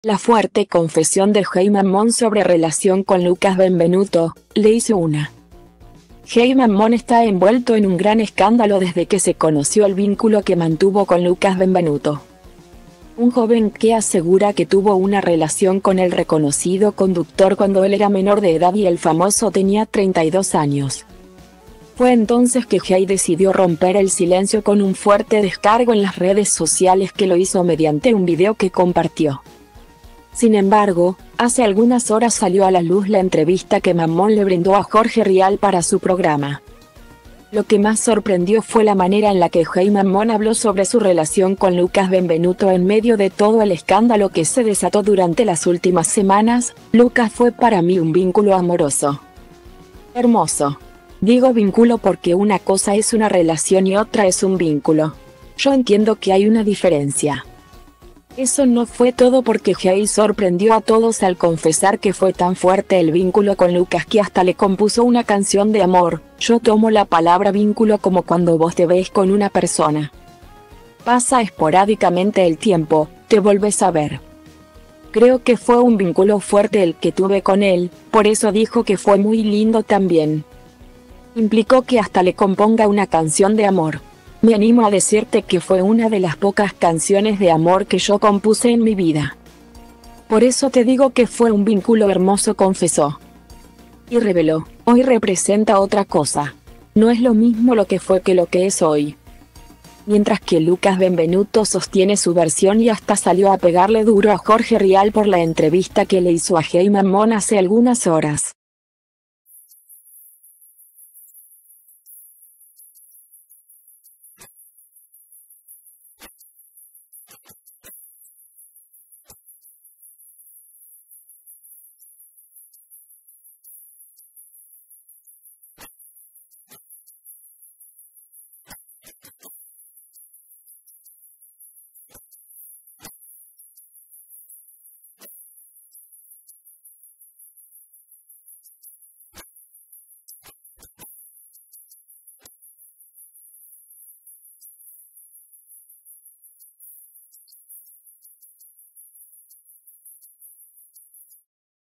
La fuerte confesión de Jey Mammón sobre relación con Lucas Benvenuto: "Le hice una…"
Jey Mammón está envuelto en un gran escándalo desde que se conoció el vínculo que mantuvo con Lucas Benvenuto,un joven que asegura que tuvo una relación con el reconocido conductor cuando él era menor de edad y el famoso tenía 32 años.